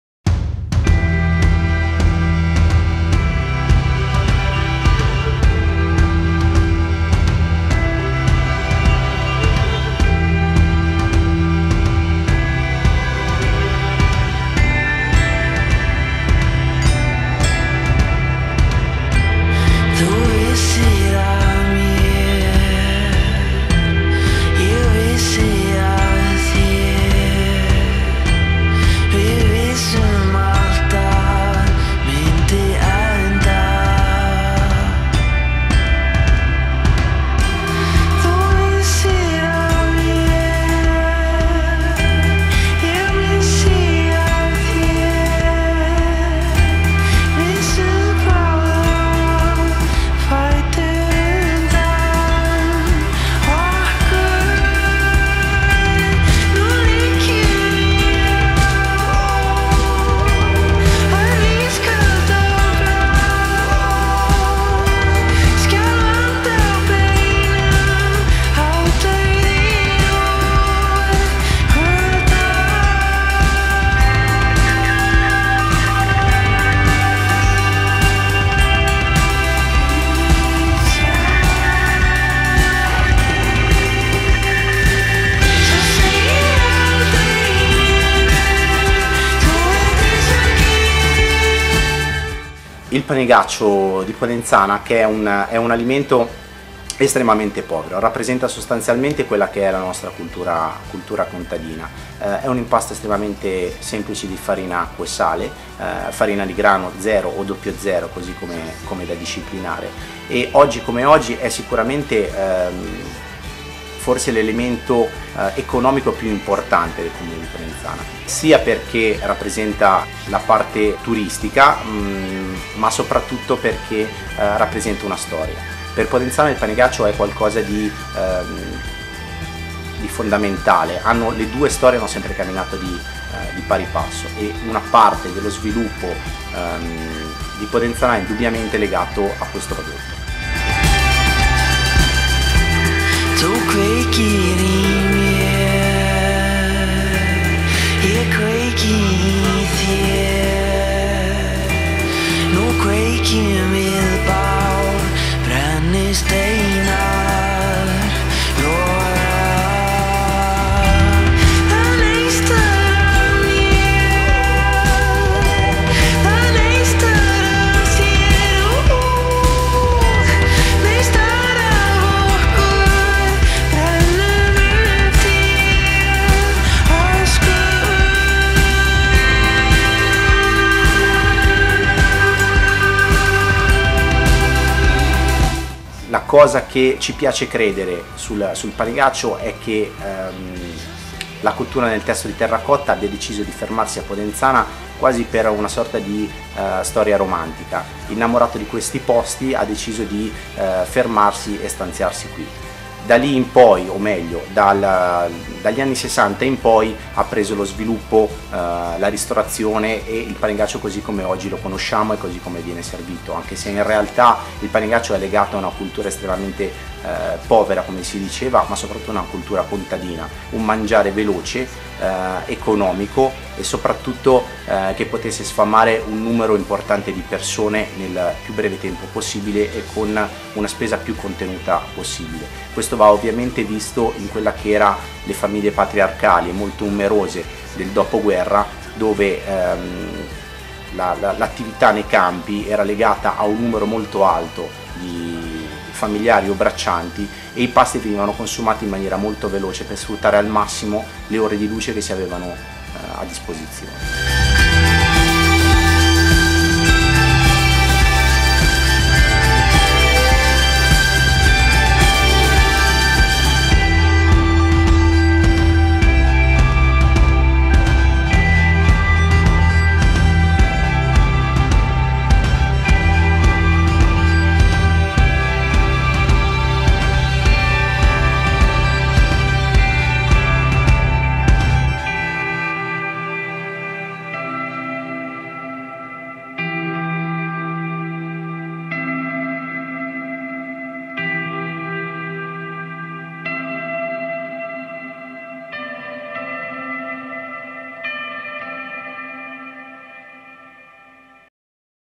Il Panigaccio di Podenzana secondo la ricetta originale
Il Ristorante da Gambin è lo storico punto di riferimento per gli amanti del vero Panigaccio di Podenzana garantito dall'ARPP.